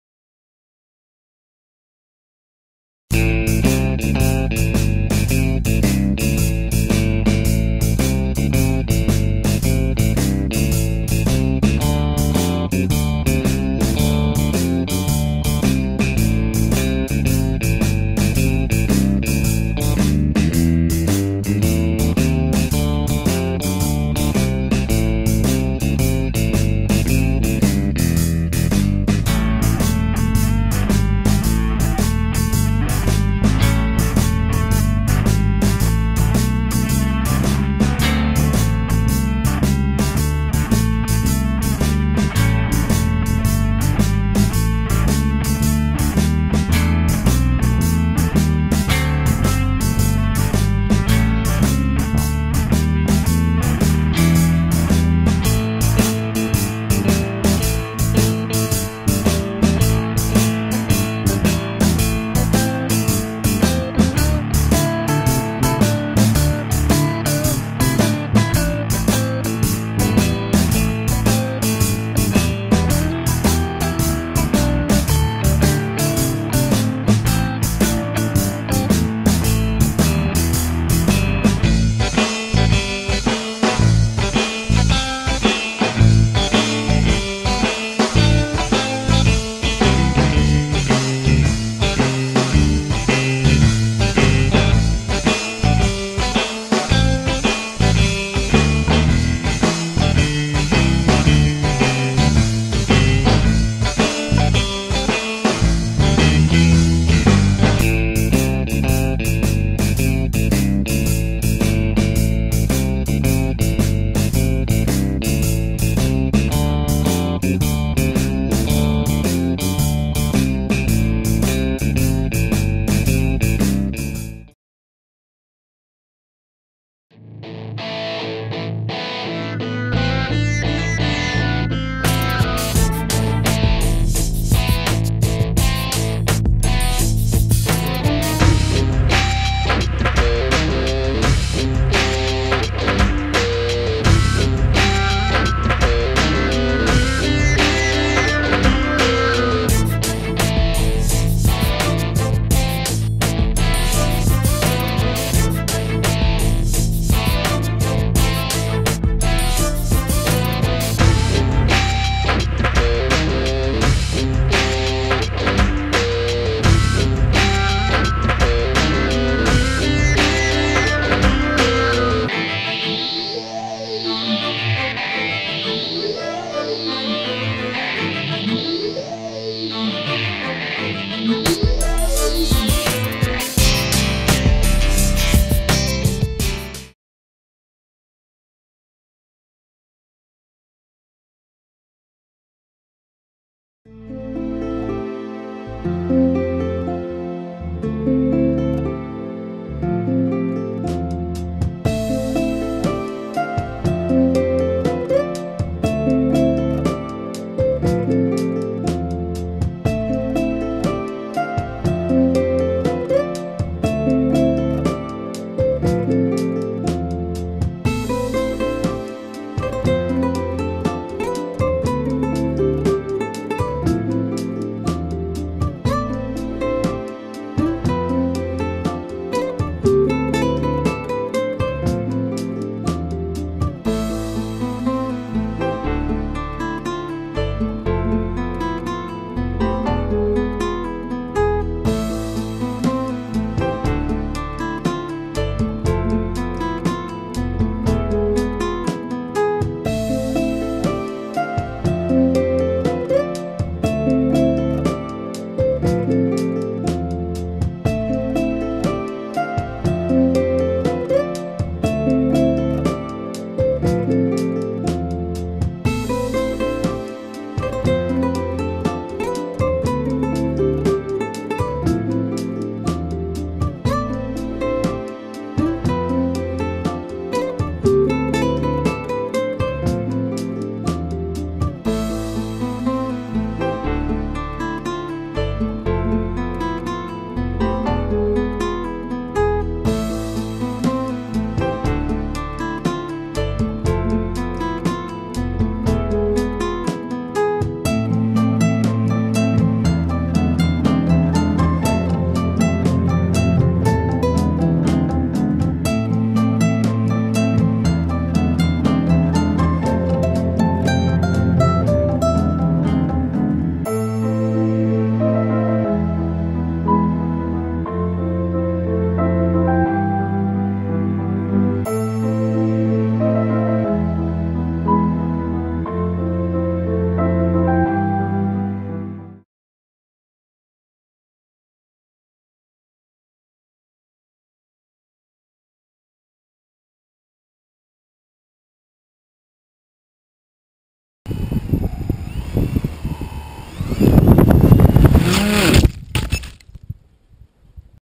Jämi RC in 25-27.4.2014 - tunnelmia ja FPV lennätystä
Kiitos järjestäjille, oli jälleen hieno ja turvallinen kokemus!